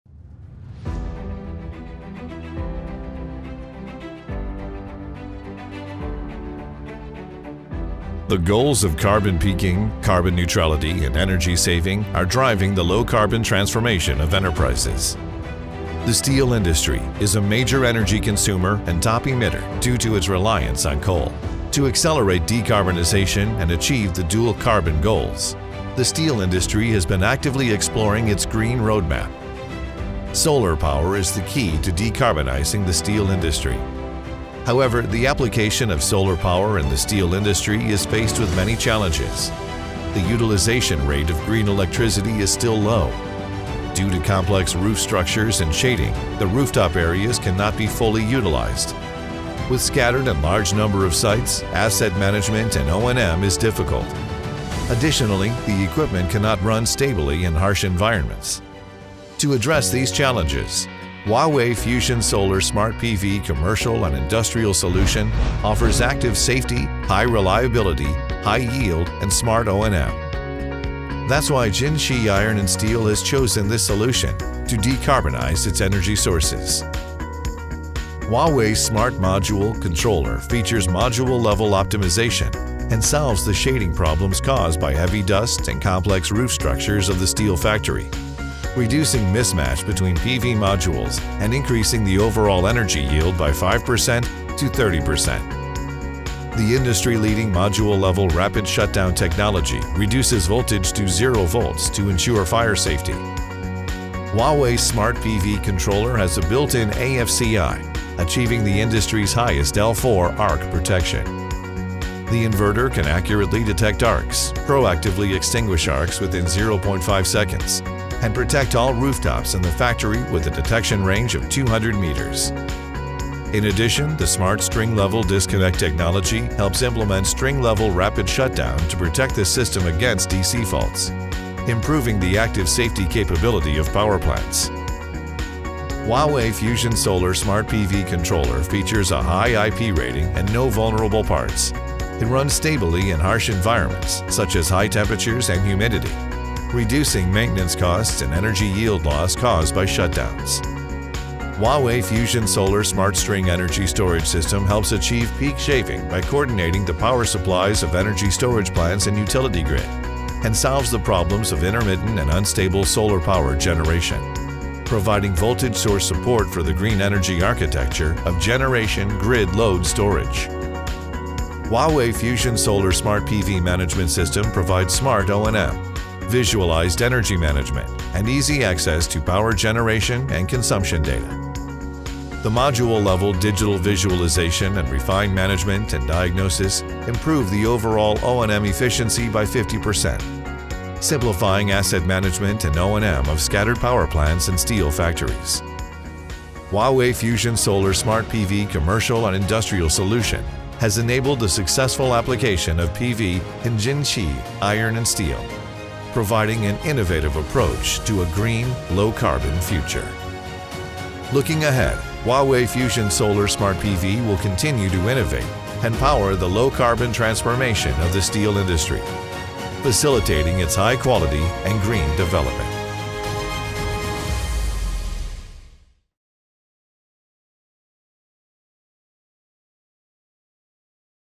PV + Steel approach to sustainability
The Huawei FusionSolar Smart PV Commercial & Industrial solution offers 4 key benefits: ActiveSafety, HighReliability, OptimalElectricityCost, SmartOandM, which helps Jinxi Steel Plant successfully transitioned to a low-carbon operation, demonstrating the effectiveness of the 'PV + Steel' sustainability model. Notably, the solution has delivered avg. annual energy yield of 50.4M kWh, saving 35M CNY/yr.